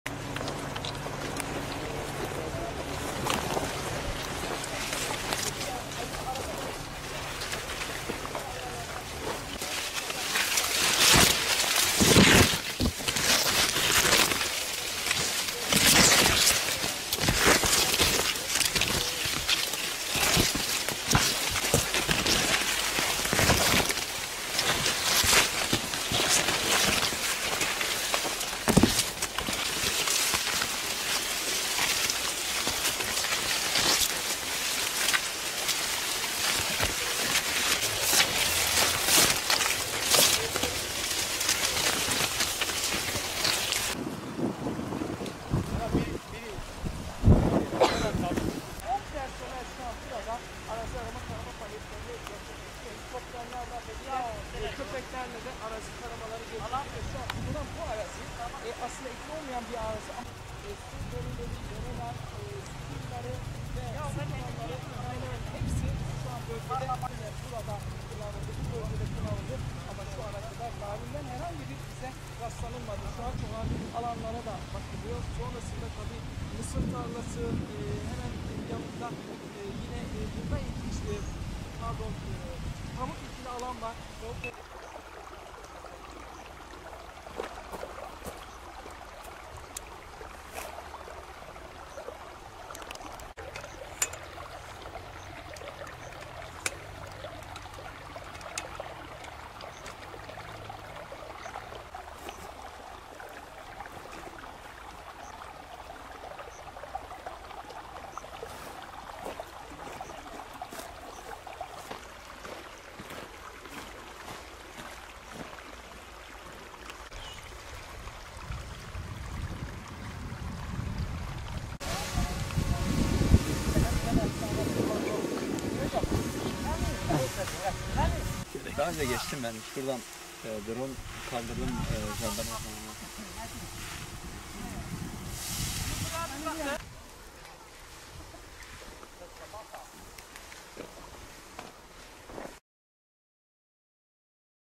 Kayıp Narin'i arama çalışmaları sürüyor: Amcası ve mahalle muhtarı gözaltına alındı
Diyarbakır’da kayıp Narin Güran soruşturması kapsamında amcası ve aynı zamanda Tavşantepe Mahallesi muhtarı S.G. gözaltına alındı.